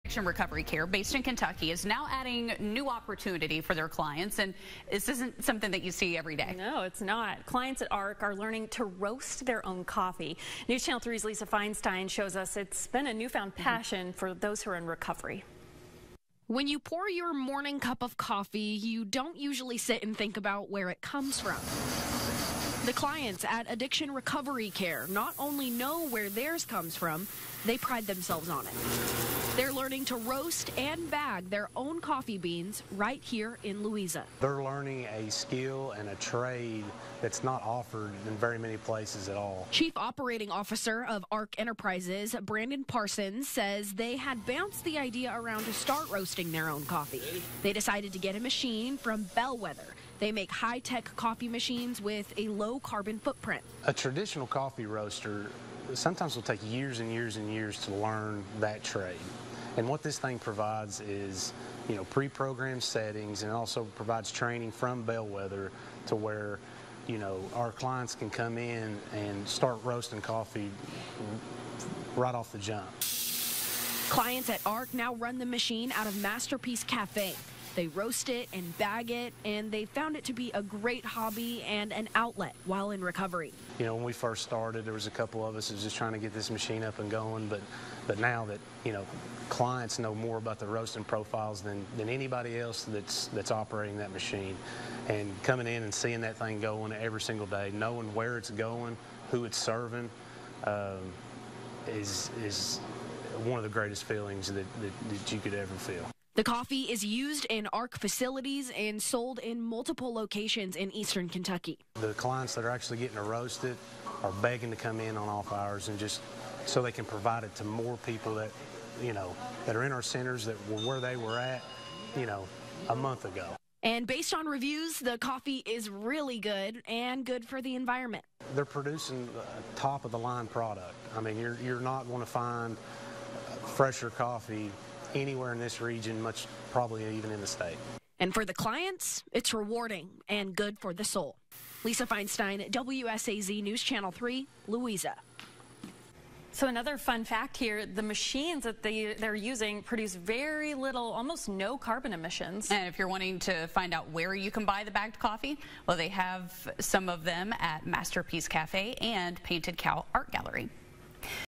Addiction Recovery Care clients find outlet in roasting their own coffee
Clients at Addiction Recovery Care are learning to roast their own coffee.